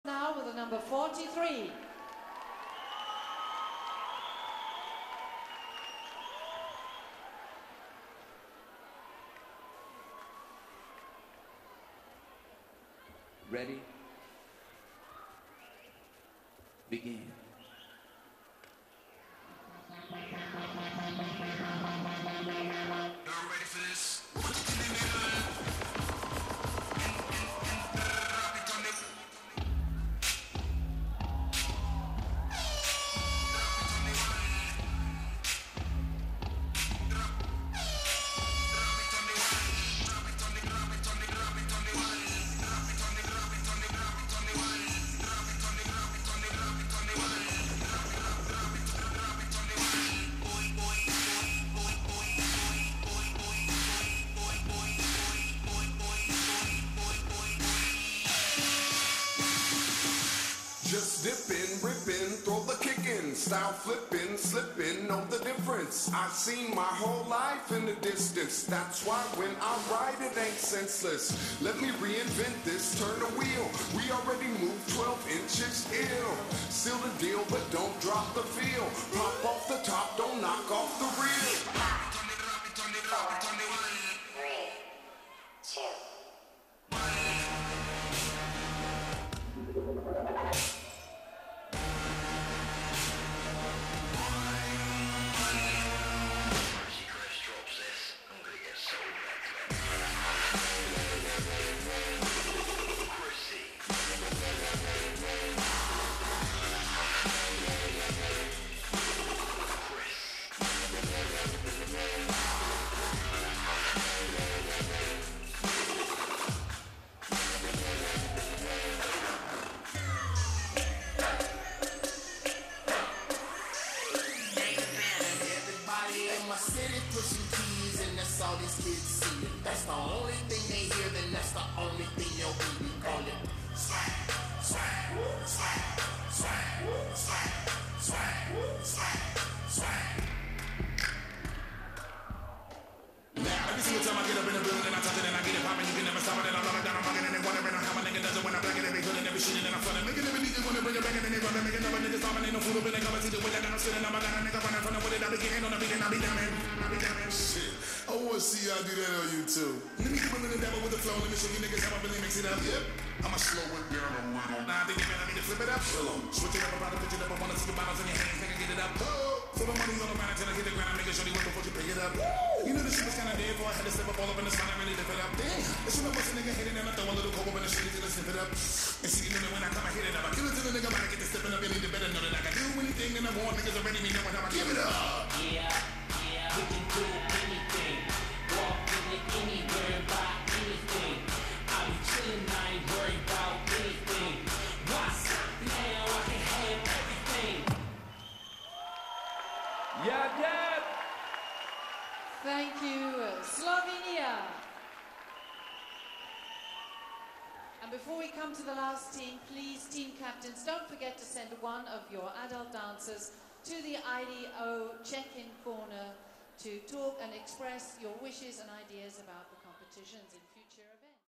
IDO Hip-Hop World Championships 2012 Bochum - "Bolero - Why Stop Now" SLOVENIA
Event: IDO Hip-Hop World Championships 2012
Place: RuhrCongress Bochum,Germany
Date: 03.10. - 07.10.
Formation: "Bolero - Why Stop Now" from Slovenia
Choreography: Žiga Mlakar
Category: Hip-Hop Formations Adults
5th Place